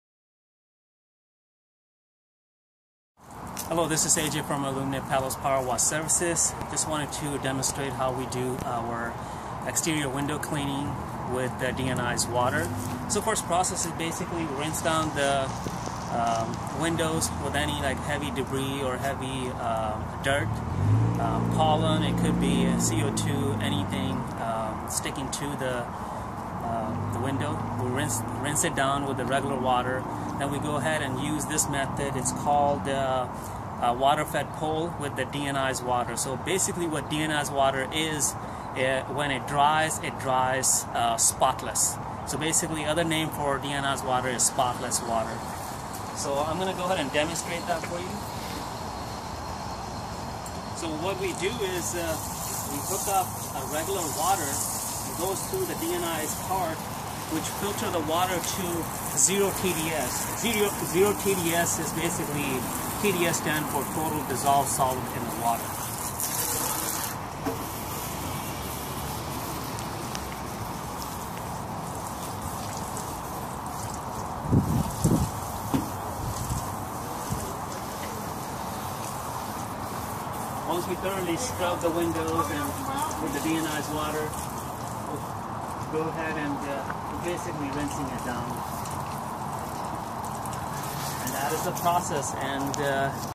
Window cleaning using Deionized water -a simple demo
Spot free windows...this is how you do it.